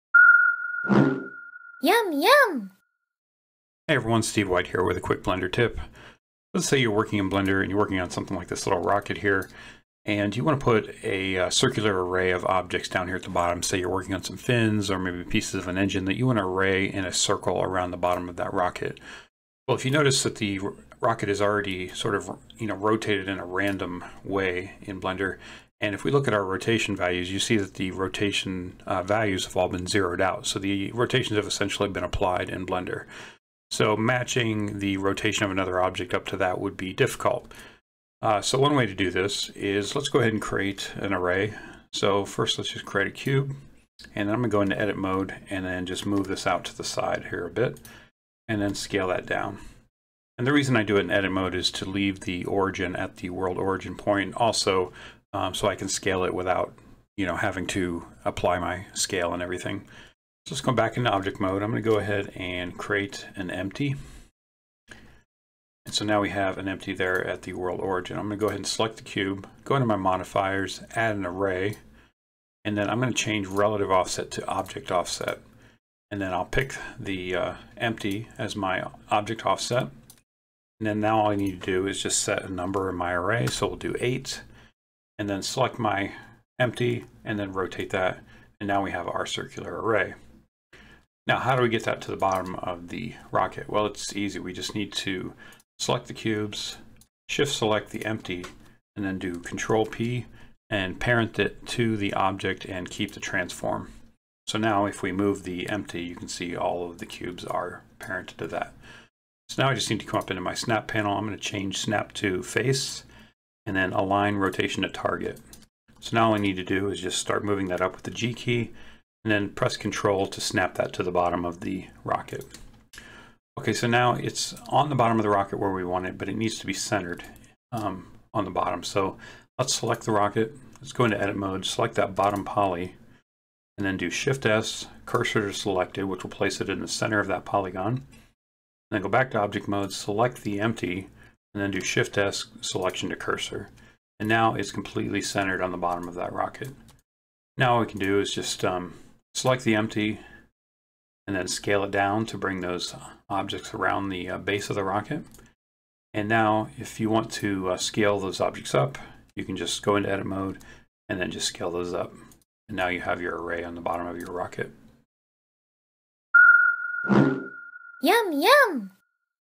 Blender Tip 001 Creating Off-Axis Arrays
In this video I show how to create a circular array on an object that has had its rotations already applied in Blender.